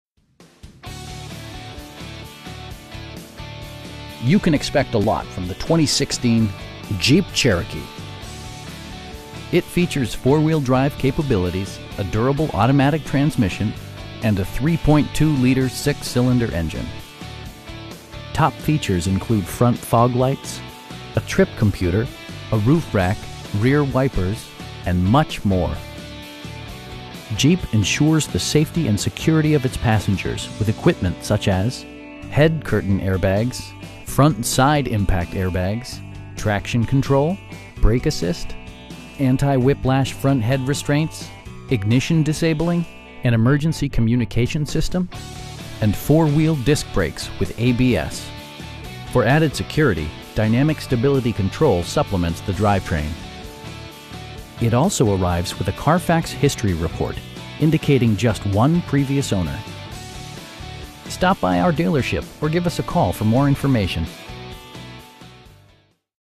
2016 Jeep Cherokee Trailh in Craig, CO 81625-3643
2705 W 1st Street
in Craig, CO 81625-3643

You can expect a lot from the 2016 Jeep Cherokee. It features four-wheel drive capabilities, a durable automatic transmission, and a 3.2 liter 6 cylinder engine. Top features include front fog lights, a trip computer, air conditioning, tilt steering wheel, cruise control, a roof rack, rear wipers, and much more. Jeep ensures the safety and security of its passengers with equipment such as: head curtain airbags, front side impact airbags, traction control, brake assist, anti-whiplash front head restraints, ignition disabling, an emergency communication system, and 4 wheel disc brakes with ABS. For added security, dynamic Stability Control supplements the drivetrain. It also arrives with a Carfax history report, indicating just one previous owner. You will have a pleasant shopping experience that is fun, informative, and never high pressured. Stop by our dealership or give us a call for more information.